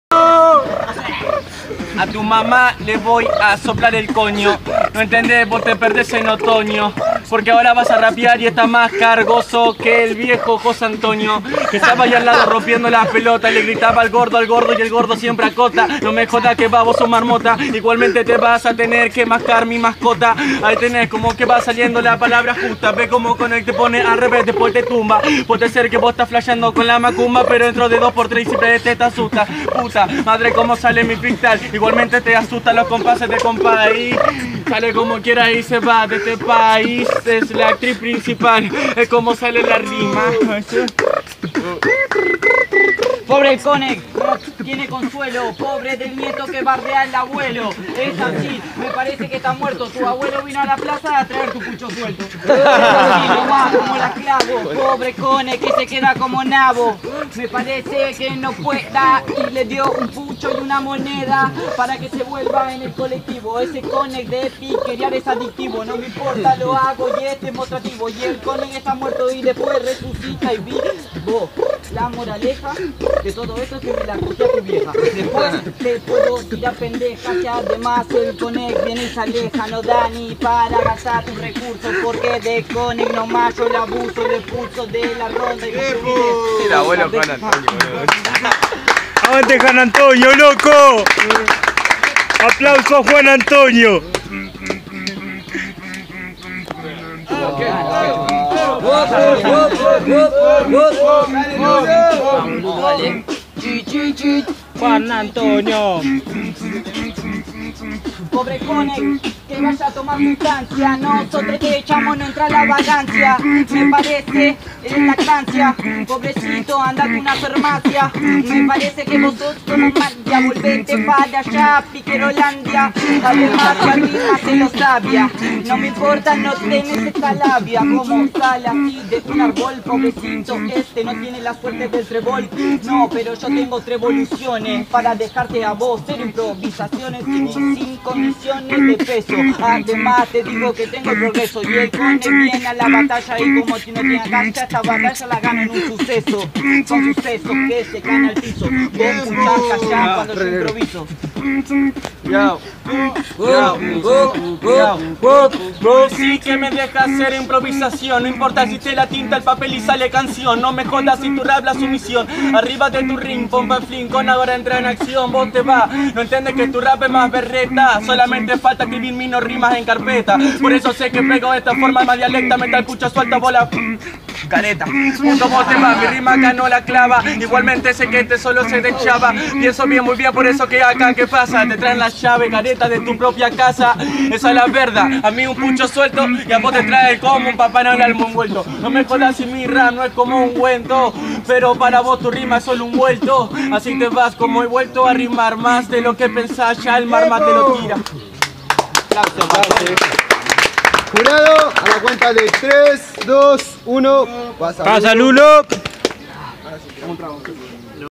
Conec vs Lulo Semifinal Skill De La Lleca Vol2
Para el que no lo sabe esta competencia va por puntos, ganas una batalla y ganas puntos, acá dejo las referencias..

Batalla ganada: 6 puntos
Batalla ganada + 1 replica: 5 puntos
Batalla ganada + 2 replicas: 5 puntos
Batalla ganada + corta bigote: 4 puntos

Si se llega a dar un triangular, van a batallar, terminados los minutos sale uno, los 2 que quedan ganaran un punto adicional..

El que se mantenga en primer lugar al finalizar el torneo sera el ganador de la under (Premio a confirmar)

Por otro lado los 8 competidores con mejor puntaje clasifican a la edición con micrófono, la cual van a ser 16 competidores, los otros 8 lugares serán disputados a través de una clasificación por temática.
Ganador: Lulo